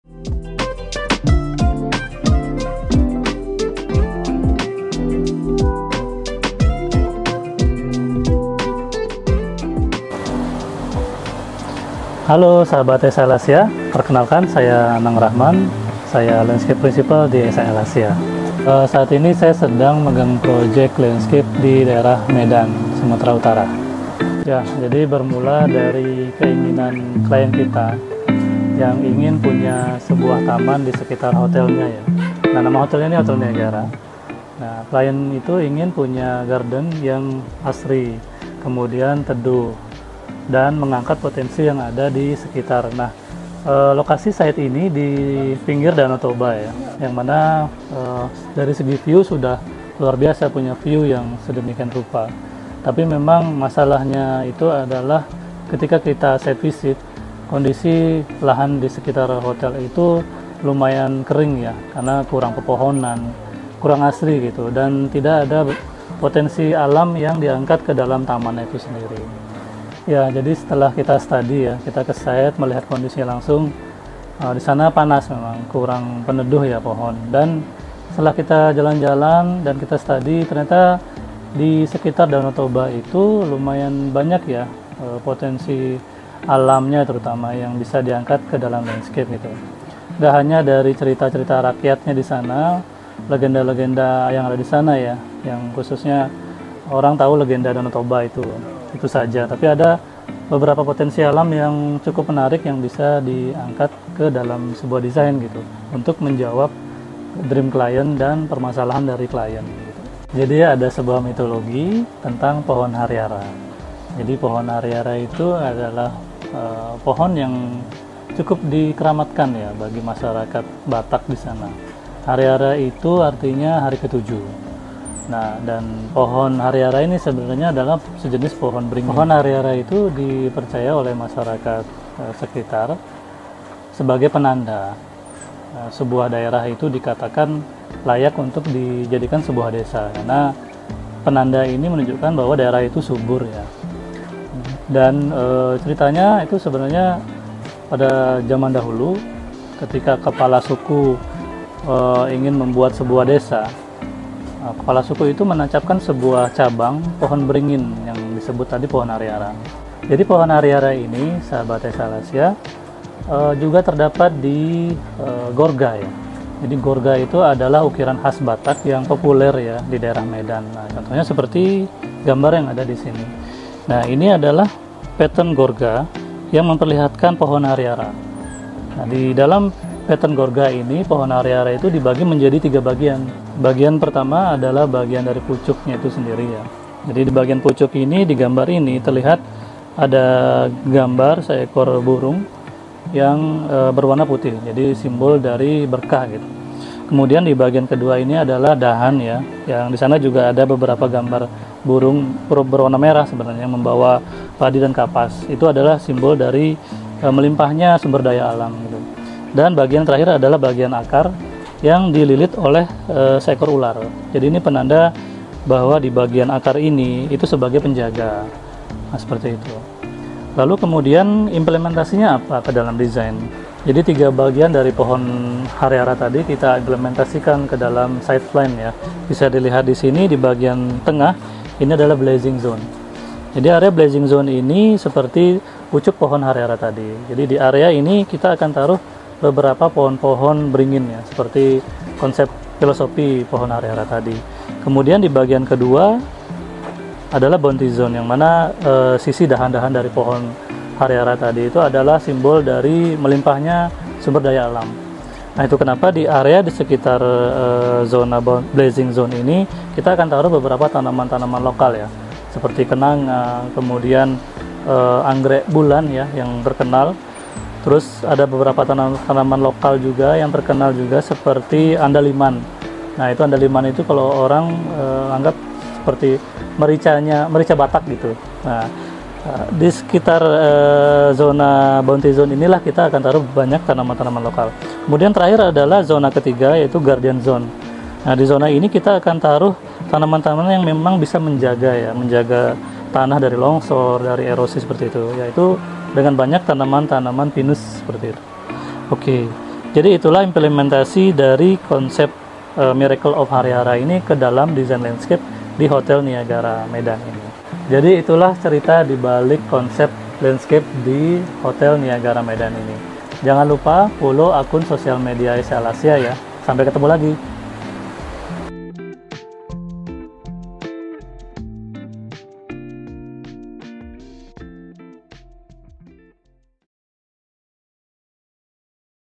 MENGIMPLEMENTASIKAN NILAI-NILAI DARI POHON YANG DISAKRALKAN DI TANAH BATAK KE DALAM DESAIN LANDSCAPE
Hai, Sahabat SHL Asia!
Pada kesempatan kali ini mas Anang Rahman, Principal Landscape di SHL Asia ingin berbagi cerita mengenai salah satu project landscape yang sedang dikerjakan di daerah Medan.